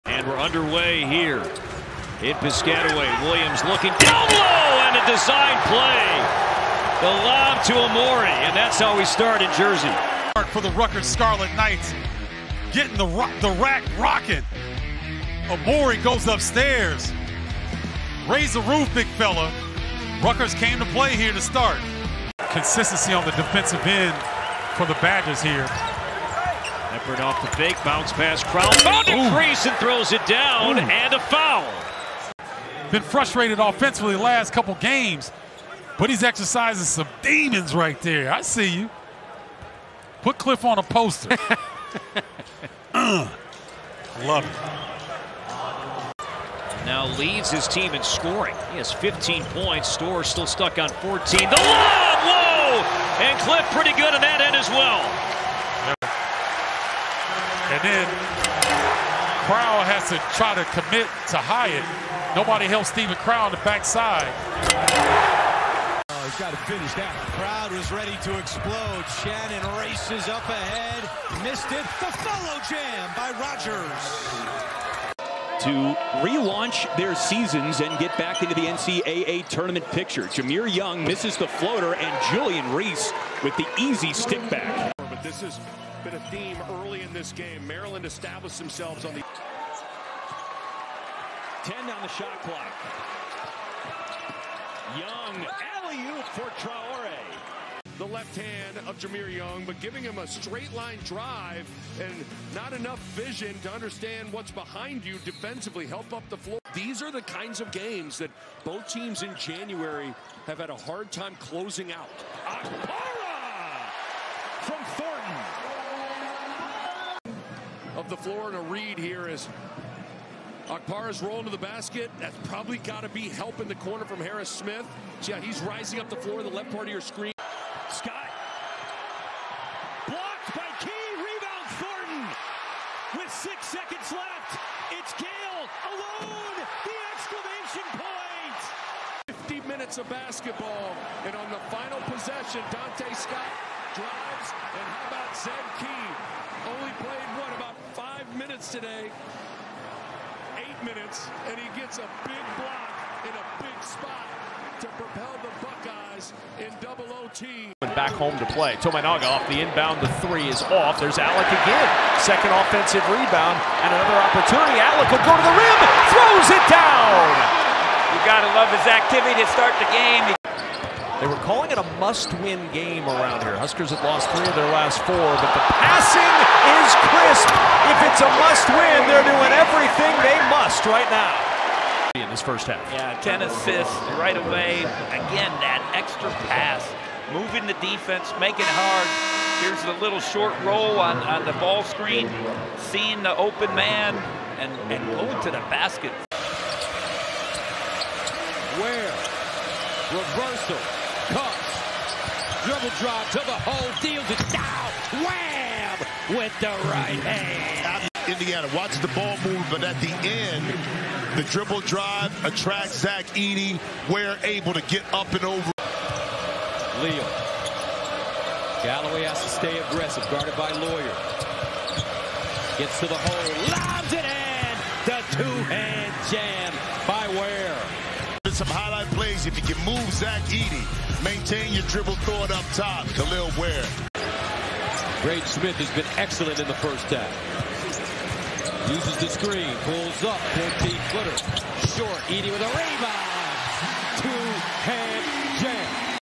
Top Dunks of the Day | Big Ten Basketball | 02/10/2024
00:00:00 - Rutgers Scarlet Knights vs. Wisconsin Badgers, 02/10/2024
00:01:06 - Michigan State Spartans vs. Illinois Fighting Illini, 02/10/2024
00:01:15 - Ohio State Buckeyes vs. Maryland Terrapins, 02/10/2024
00:02:37 - Nebraska Cornhuskers vs. Michigan Wolverines, 02/10/2024
00:03:15 - Purdue Boilermakers vs. Indiana Hoosiers, 02/10/2024